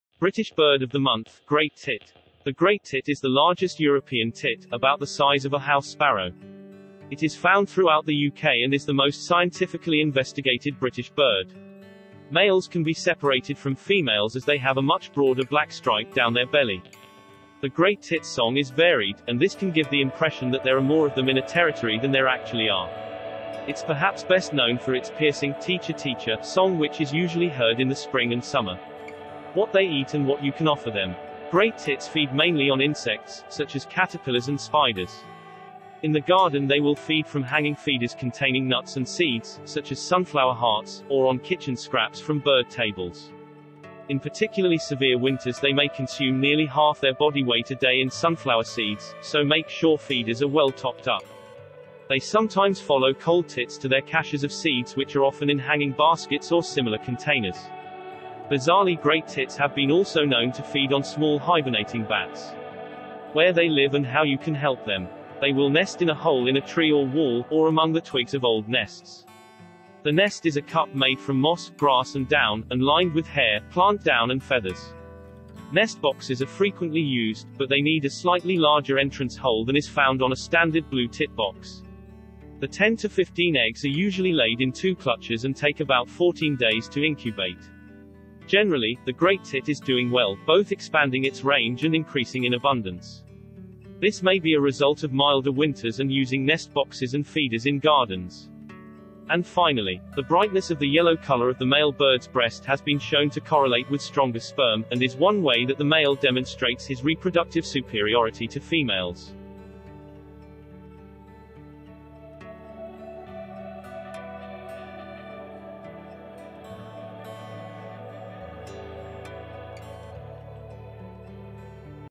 British Bird of the Month: Great Tit
The Great Tit is the largest European tit – about the size of a House Sparrow. It is found throughout the UK and is the most scientifically investigated British bird.

Males can be separated from females as they have a much broader black stripe down their belly.

The Great Tits’ song is varied, and this can give the impression that there are more of them in a territory than there actually are. It’s perhaps best known for its piercing “teecher-teecher” song which is usually heard in the spring and summer.